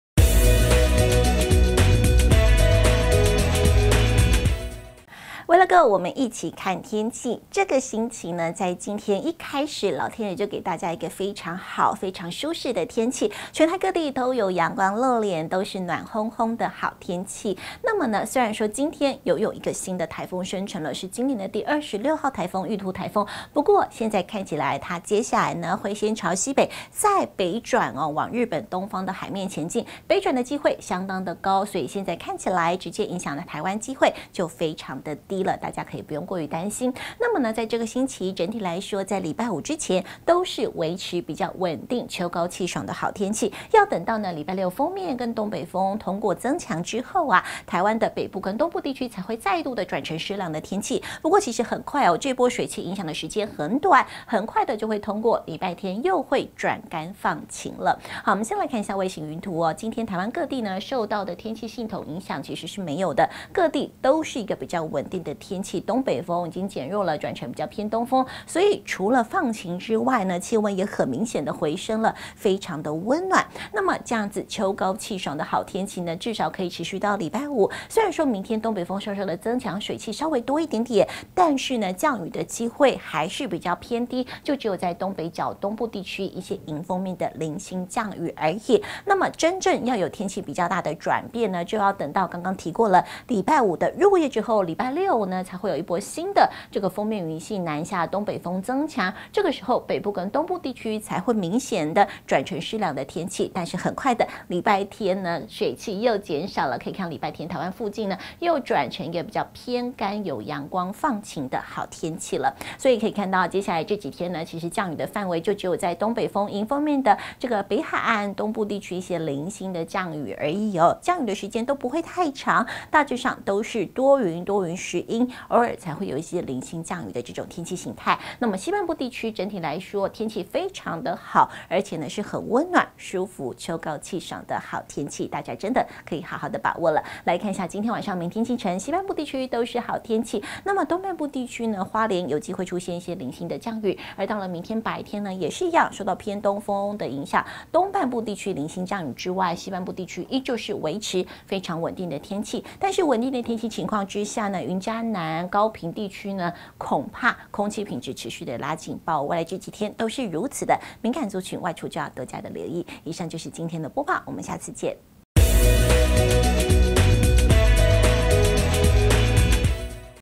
2018/10/22 玉兔颱風生成 北轉機會高 周五前大致穩定好天氣
玉兔颱風生成 北轉機會高
周五前大致穩定好天氣
周六鋒面通過 東北風增強
北東濕涼 周日轉乾放晴